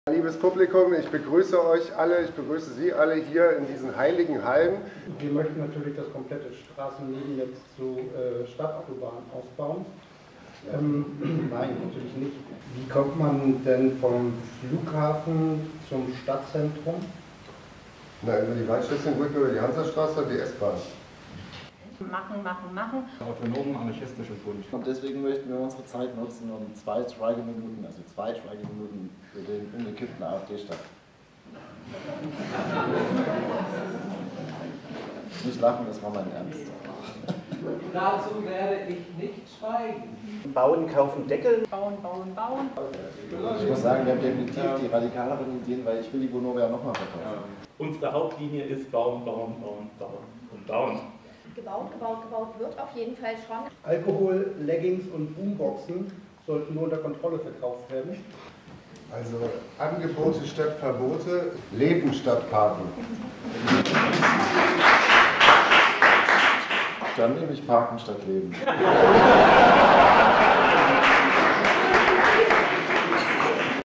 Schlagabtausch - Die Highlights (14. Mai 2019)
Wir haben zum Schlagabtausch ins projekttheater geladen. Hier könnt ihr in aller Kürze die Highlights sehen.

Zum Schlagabtausch begaben sich:
Martin Schulte-Wissermann und Jan Kossick als Gastgeber von den Piraten, Max Aschenbach und Charlotte Brock von der Partei, Caroline Lentz und Magnus Hecht von der Linken, Tina Siebeneicher und Ulla Wacker von den Grünen, Sabine Friedel und Vincent Drews aus der SPD, Benita Horst für die FDP, Johannes Alberecht Heinke und Hans-Jürgen Rosch von der CDU und Christian Hille von den Freie Bürgern.